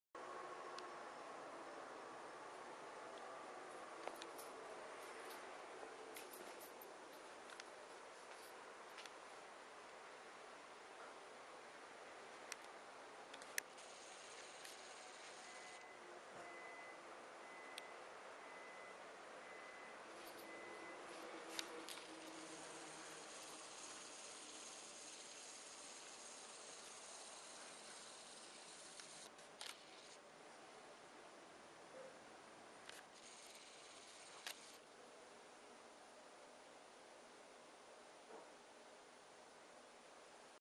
お昼寝・・チワワのイチローちゃん
少し暖かくなってきたのでこんな感じでねています。